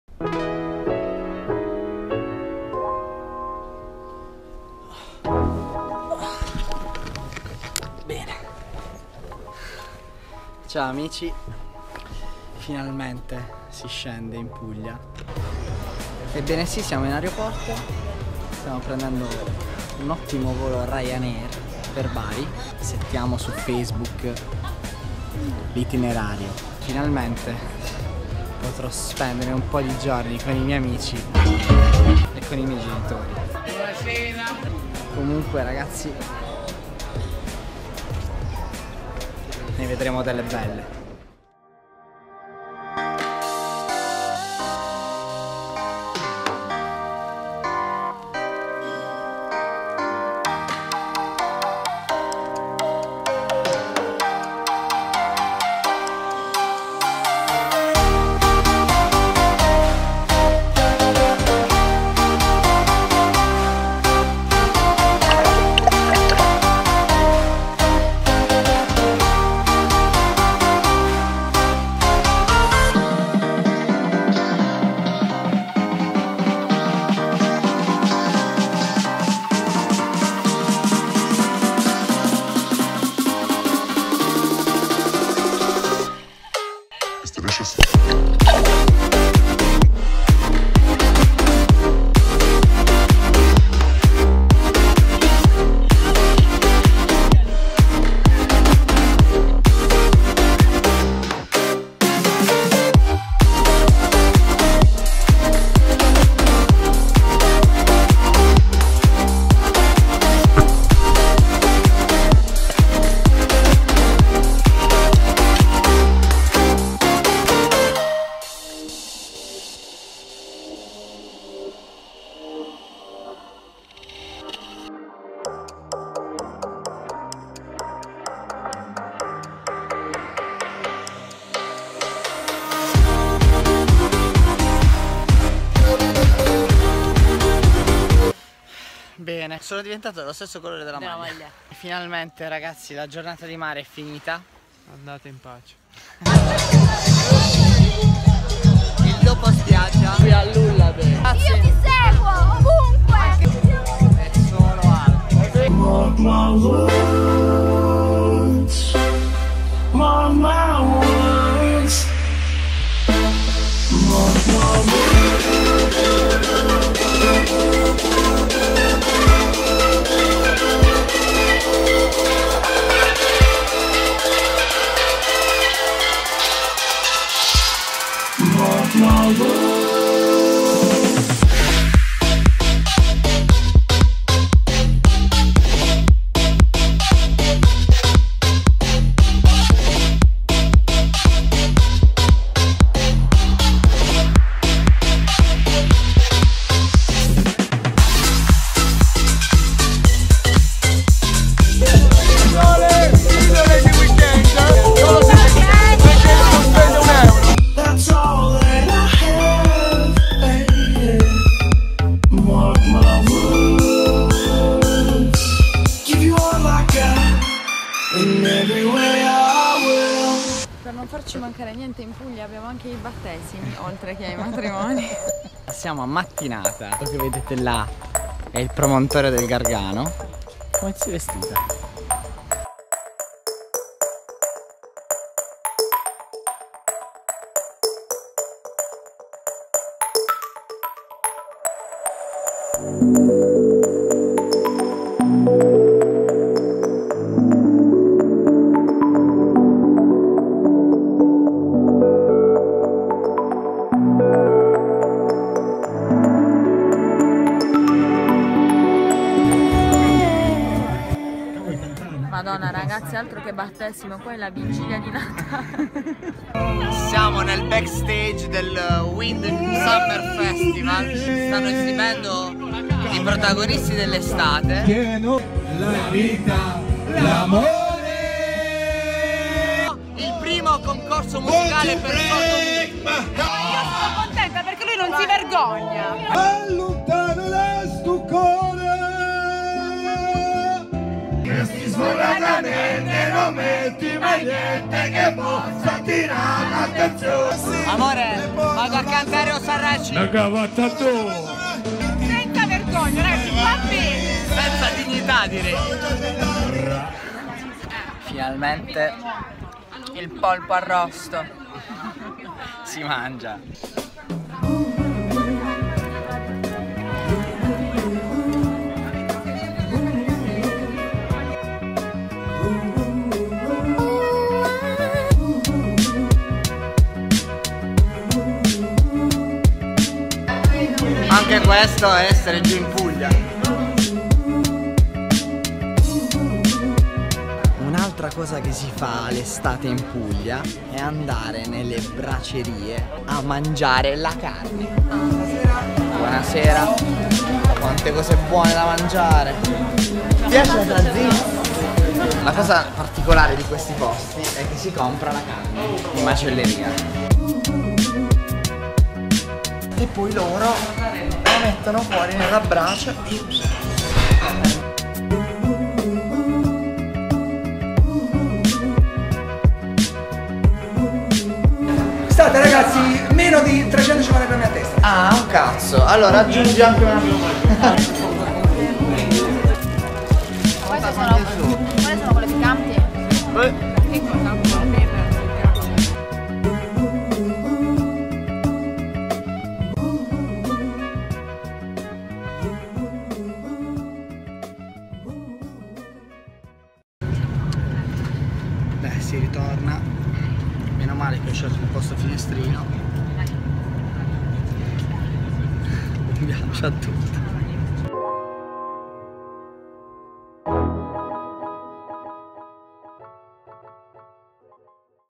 PILLOLE DI WEEKEEND #63 - VACANZE IN PUGLIA | Travel Video
Un weekend in Puglia d'Estate... ecco tutto quello che si può fare!

i posti che vedrete nel video sono Torre a Mare (BA), Capitolo (BA), Torre Canne (BR), Rosa Marina (BR), Porto Cesareo (LE) e Mattinata (FG)

Grazie mille

Pillole di Weekend